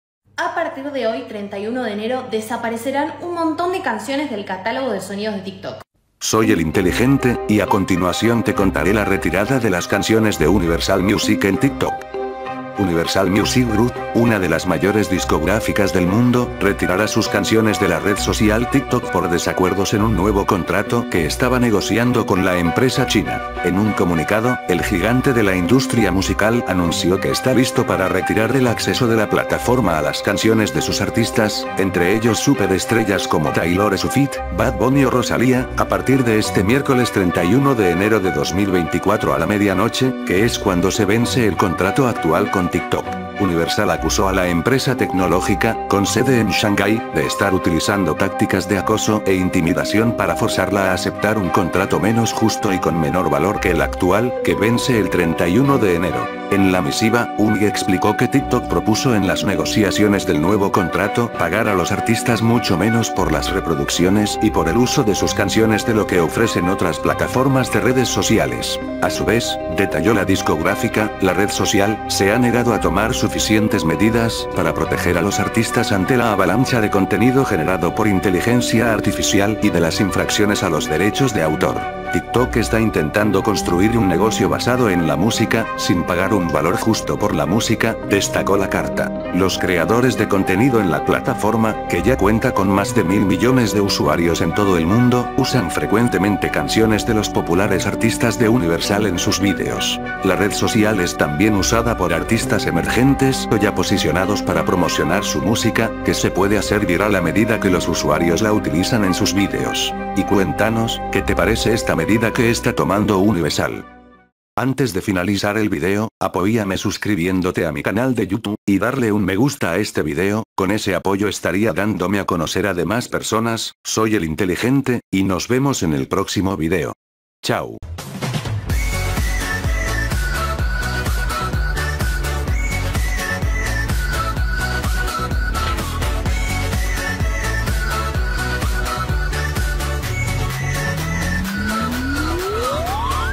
Universal Music retirará sus canciones de TikTok por desacuerdos con la plataforma • El Inteligente
Universal Music retirará sus canciones de TikTok por desacuerdos con la plataforma. Universal Music Group (UMG), una de las mayores discográficas del mundo, retirará sus canciones de la red social TikTok por desacuerdos en un nuevo contrato que estaba negociando con la empresa china.

El Inteligente ©